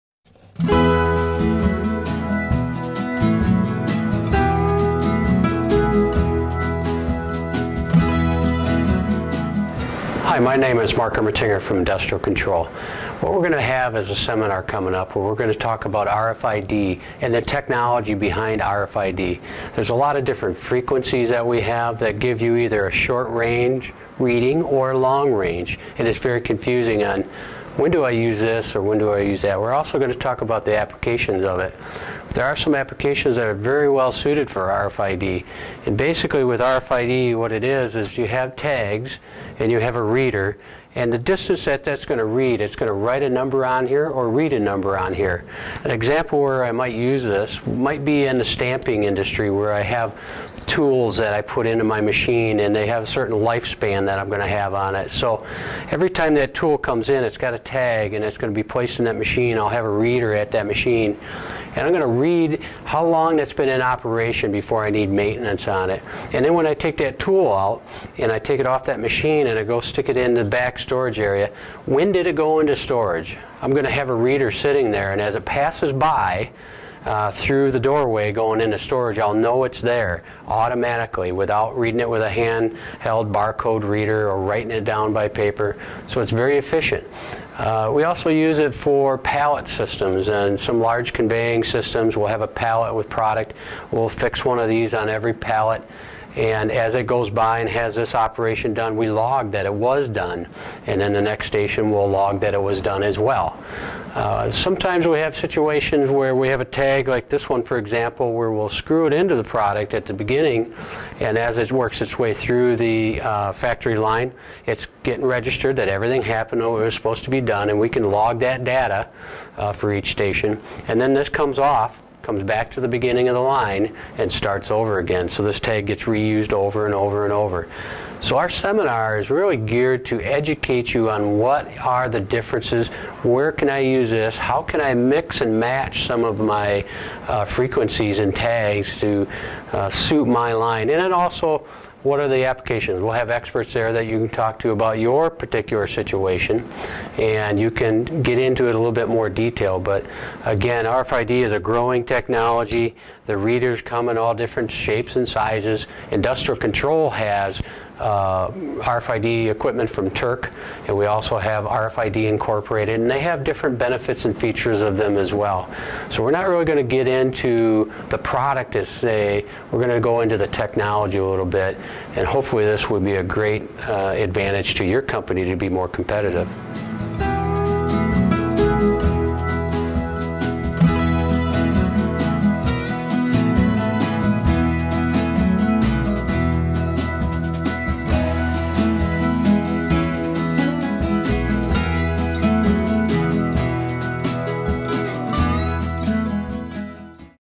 Turck RFID demo
TURCKs BL ident RFID system is the first modular RFID system with built-in I/O on the market. Plus, BL ident can be integrated into existing platforms, and supports PROFIBUS®-DP, DeviceNet™, Modbus-TCP, PROFINET and EtherNet/IP™. The simulator can help you design a system tailor-made to your specific application needs.